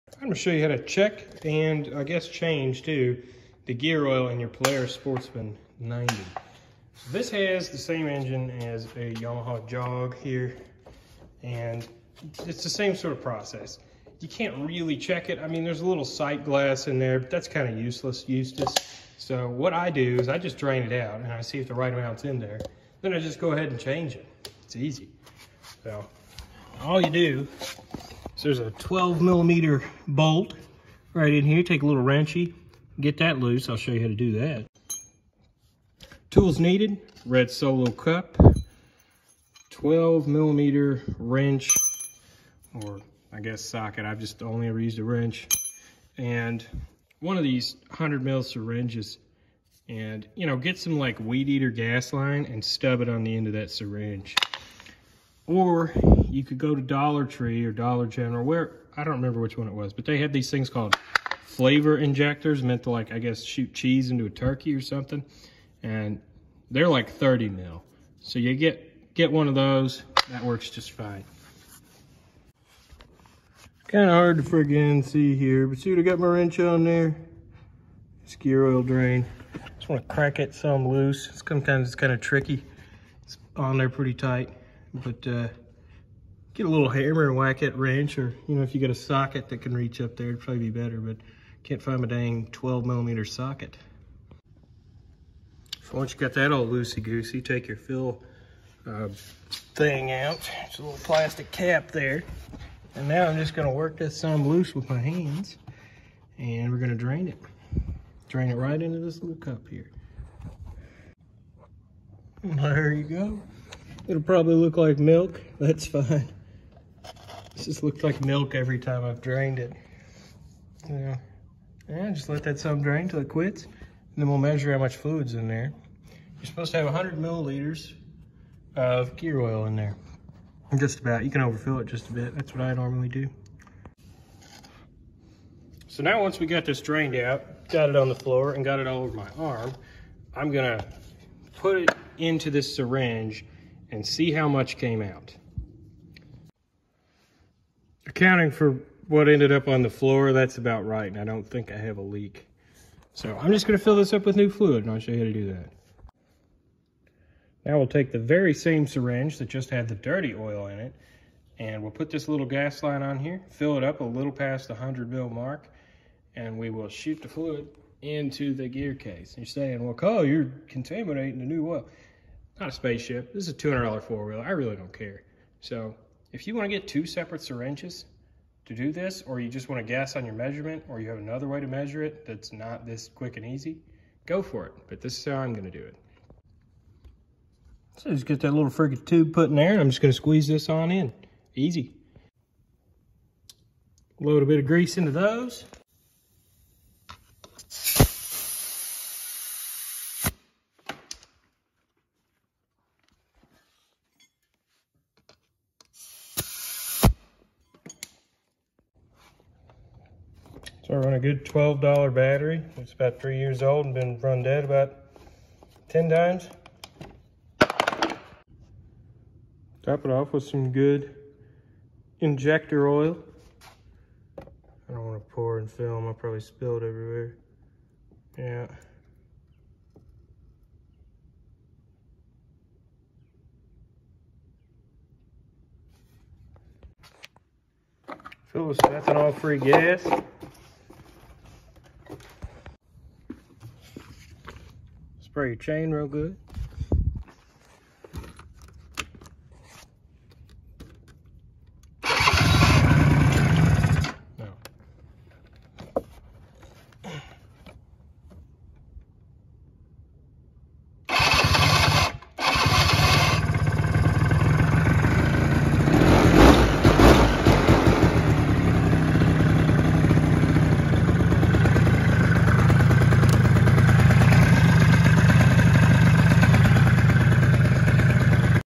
2001-2006 Polaris Sportsman 90 gear oil change and check
Easy stuff chef